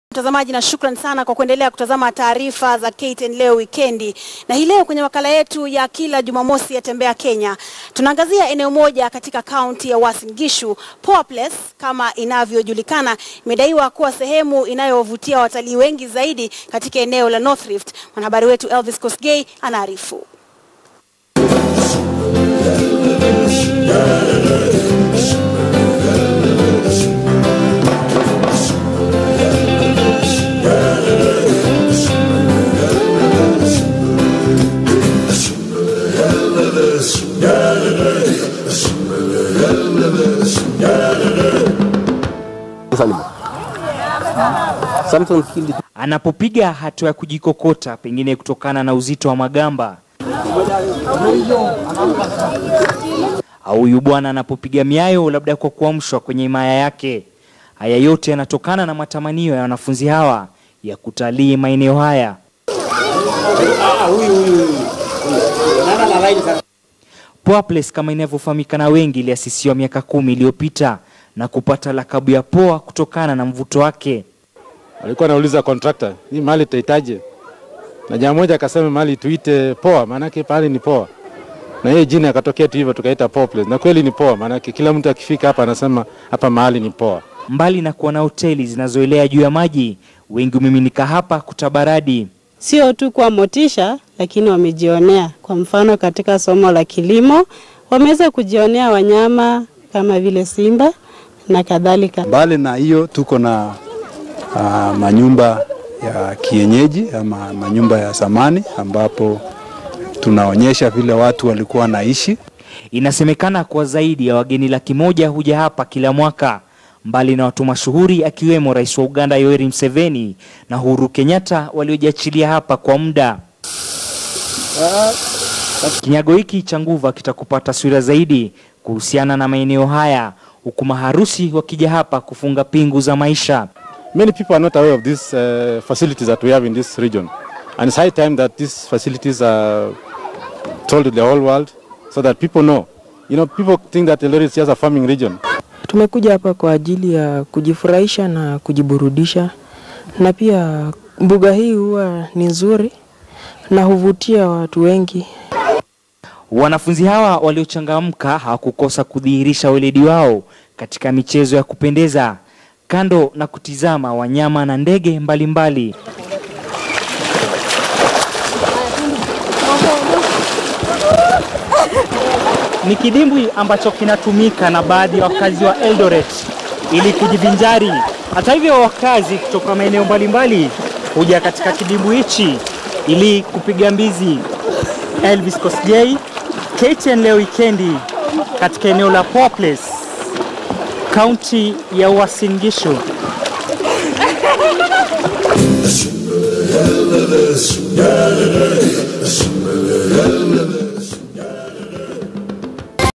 Tembea Kenya: Tunaangazia Poa Place, Uasin Gishu county
Hii leo kwenye makala yetu ya kila jumamosi ya Tembea Kenya, tunaangazia eneo moja katika kaunti ya Uasin Gishu. Poa Place kama ijulikanavyo imedaiwa kuwa sehemu inayowavutia watalii  wengi zaidi katika eneo la North Rift. Elvis Kosgei anaarifu.